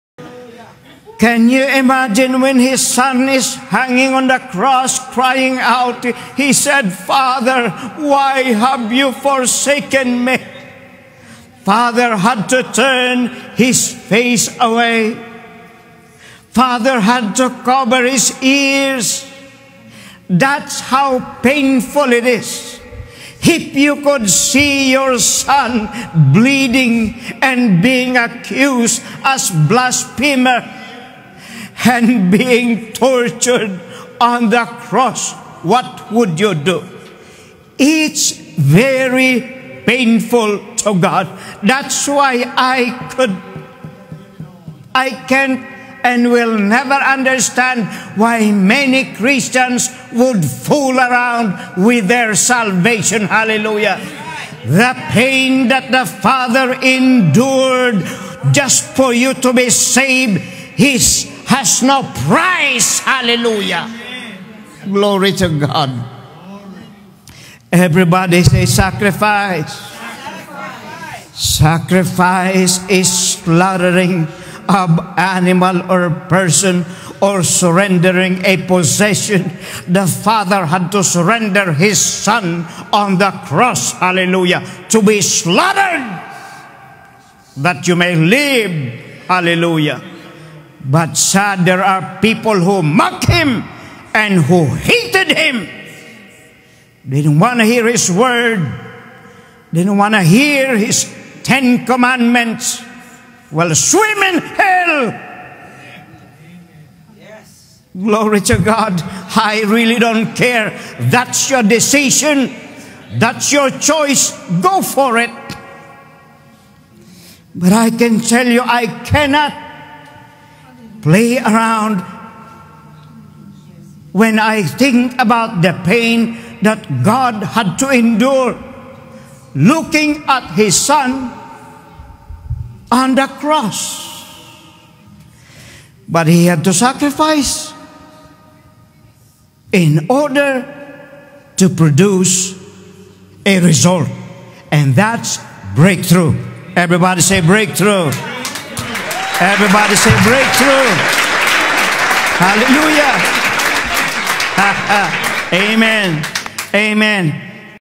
Breakthrough Must Be Taken By Force - Bishop Jerry Pena
The pain that God, our Father, has to endure while looking at His Son hanging on the cross is priceless. He went through pain so that you and I could be save from the fires of hell.